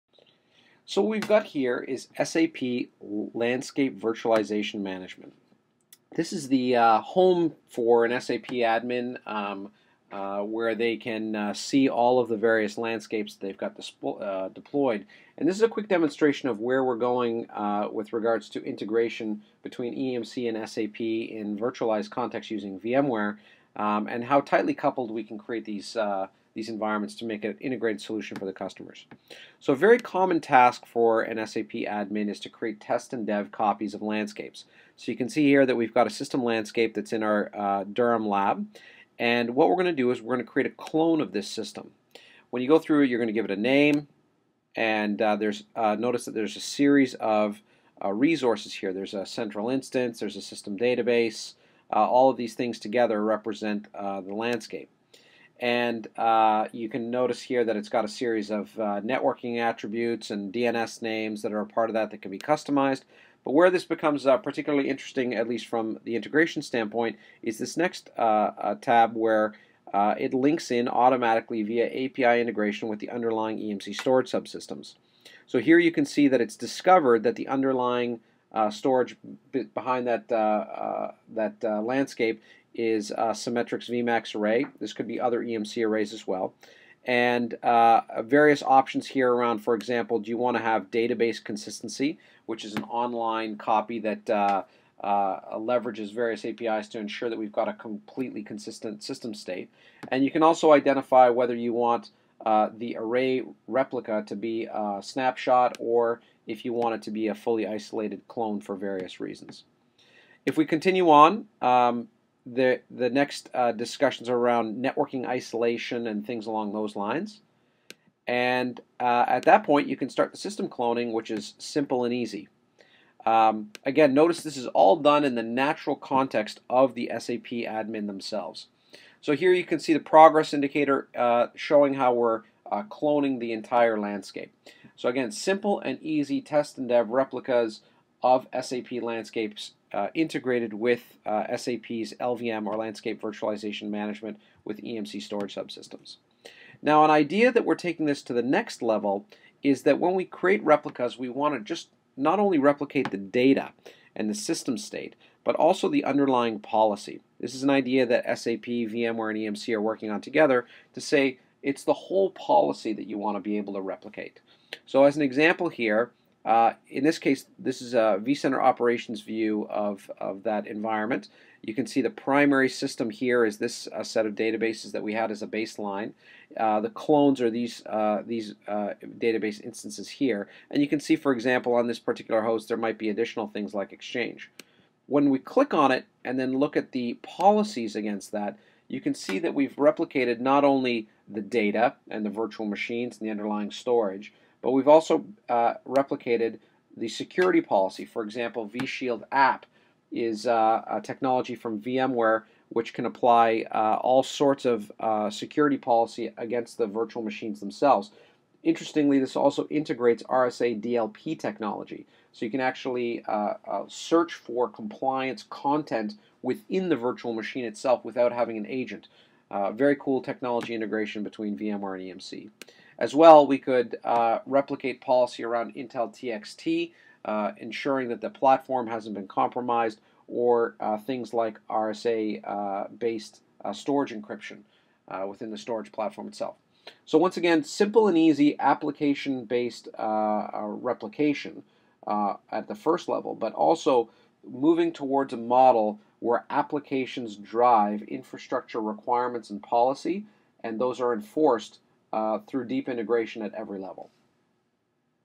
SAP LVM and EMC Integration Demo from EMC World 2012
This a demonstration of a pre-release version of SAP Landscape Virtualization Management integration with EMC Storage.   This integration makes cloning test and dev (and for other purposes) entire SAP Landscapes, and does it in the native management tools of SAP - but tightly integrated with EMC and VMware technologies.   Furthermore, this demonstration provides a view further out, where EMC is working to not only replicate the data, but the policy around the application as part of the process.